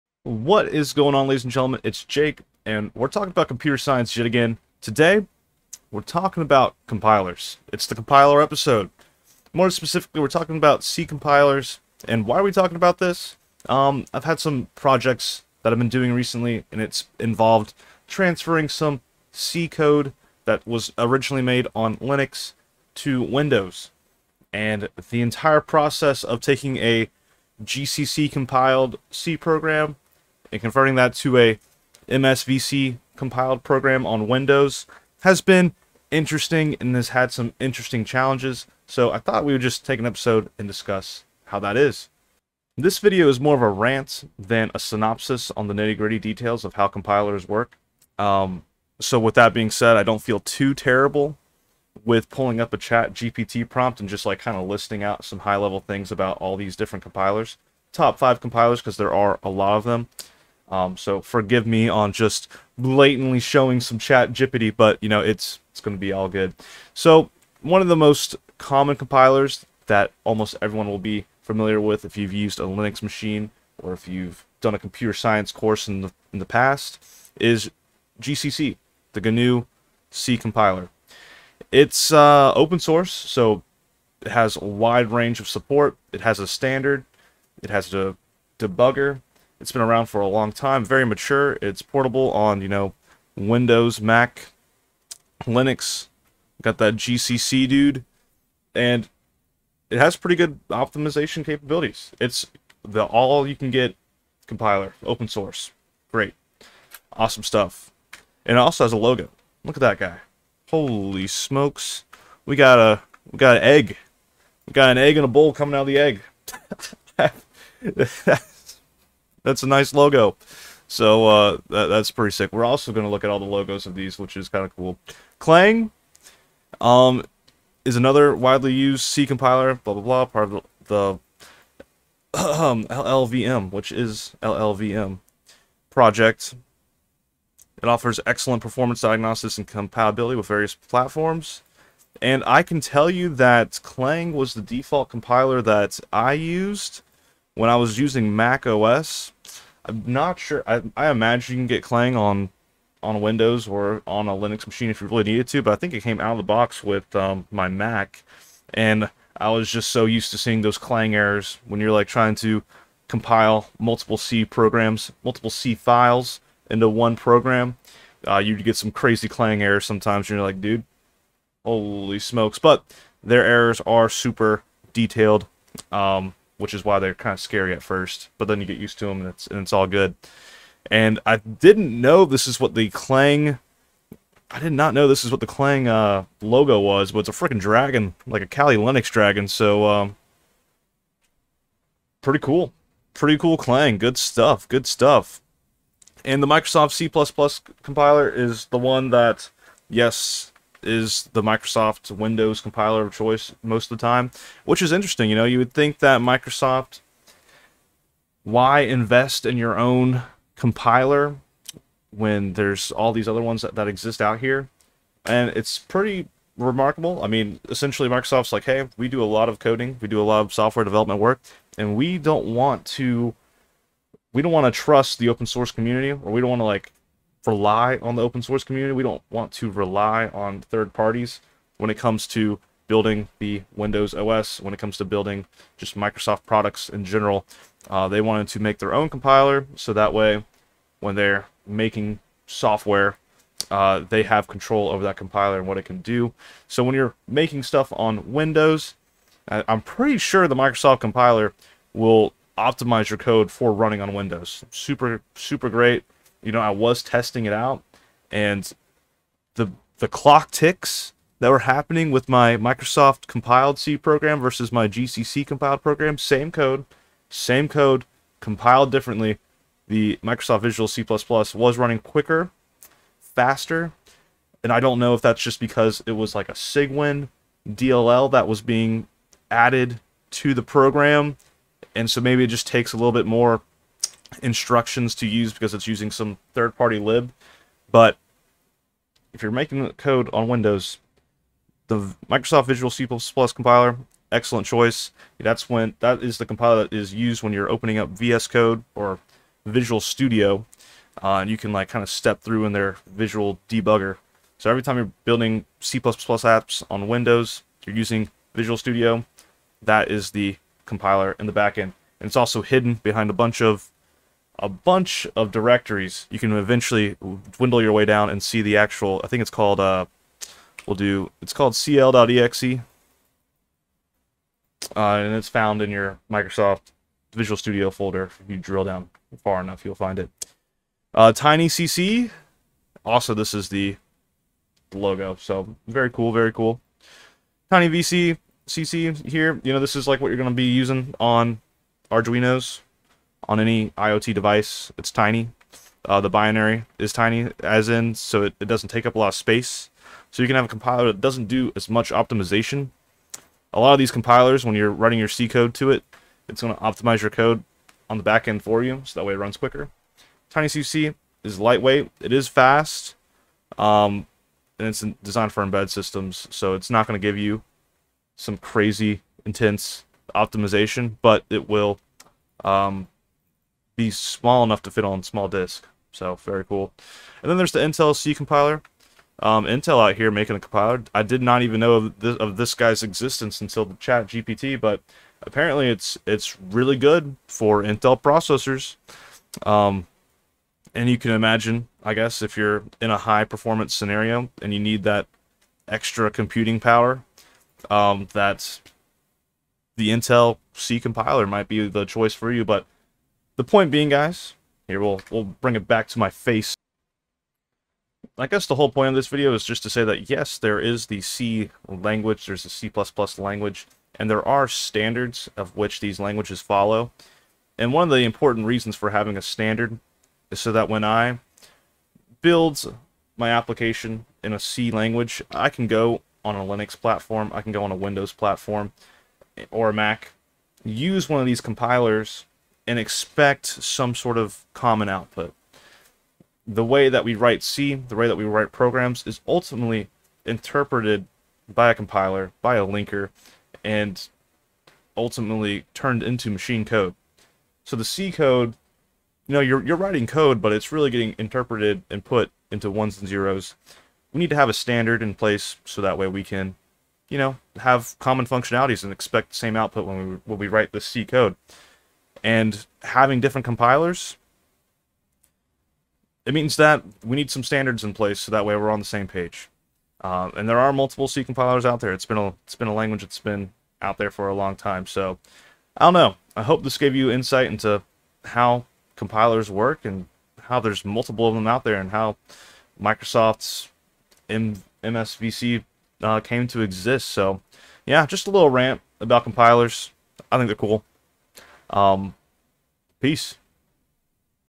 Best C Compilers (And Why They are Different!)
Hello Youtube, In this video we navigate through the top 5 most used C compilers and how they are different from One another and how they came to exist! More of a quick chat than deep technical analysis. Enjoy!